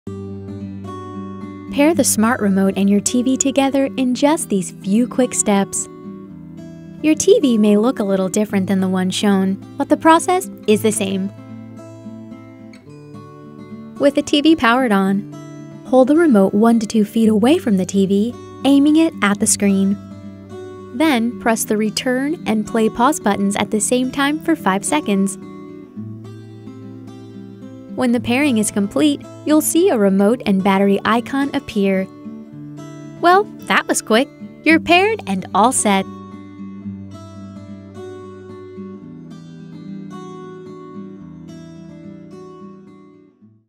How to pair the Smart Remote to your TV | Samsung US
Your TV and remote should have automatically paired together when you first took them out of the box, however if that wasn’t the case or if the connection was lost, you can manually pair the two together easily. In this video you’ll get the inside scoop on how to pair your remote to your TV. A helpful tip: before trying to repair the two, double check the batteries and make sure they are good!

If your Samsung remote is not working you can read more about how to perform basic troubleshooting